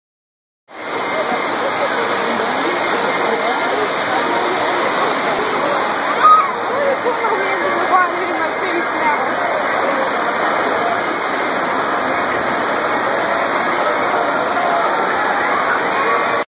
Gully Washer
Kevin, Mindy and Riley ride the Gully Washer